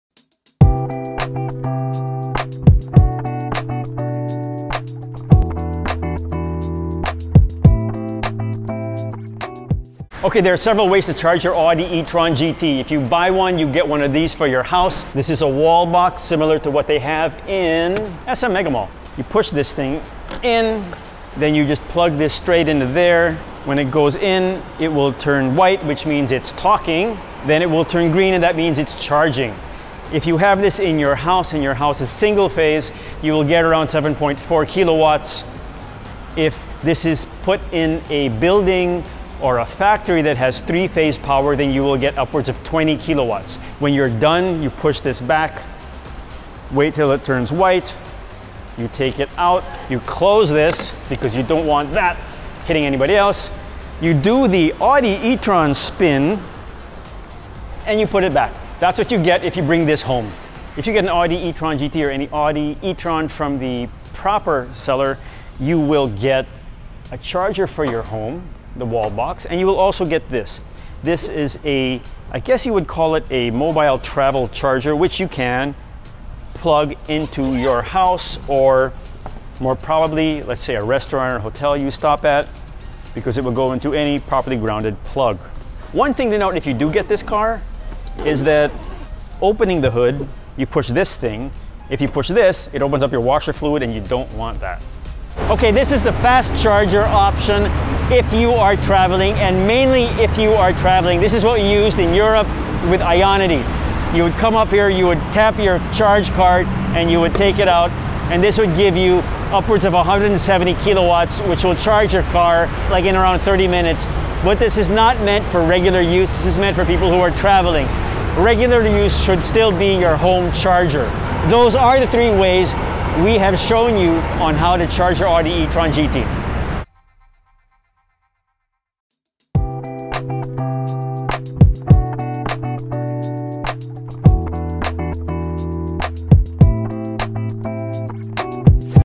Audi e-tron GT: How do you actually charge an electric car?
There are different ways to charge your electric car, and they aren't always what you may think. Studies show 90% of charging is done at home, and that is actually best. Fast chargers are more for travel top-ups.